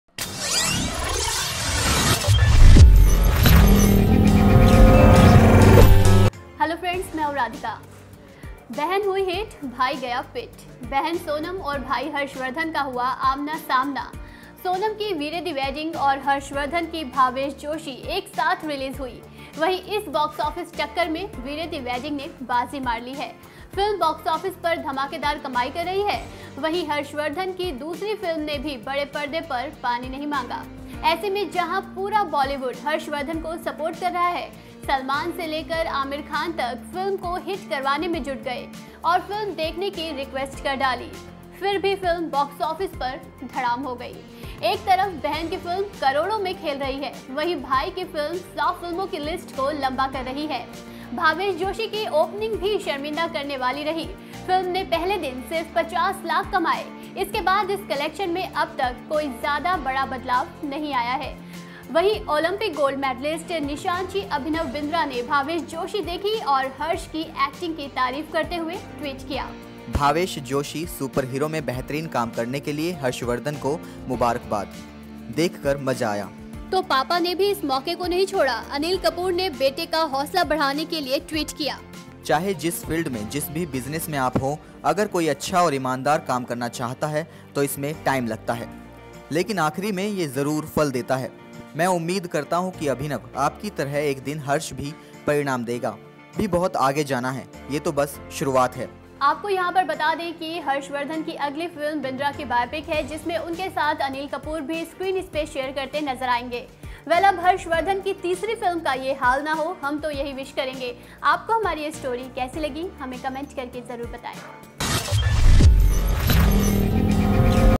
Harshvardhan की Film Bhavesh Joshi पिट गई, Anil Kapoor ने झक्कास अंदाज में बढ़ाया हौसला
Sonam की Veere Di Wedding और Harshwardhan की Bhavesh Joshi एक साथ Release हुई। वहीं इस Boxoffice टक्कर में Veere Di Wedding ने बाजी मार ली है। ऐसे में अब पापा Anil Kapoor ने बेटे Harshwardhan का बढ़ाया हौसला।

producer- Radhika
edit- shrestha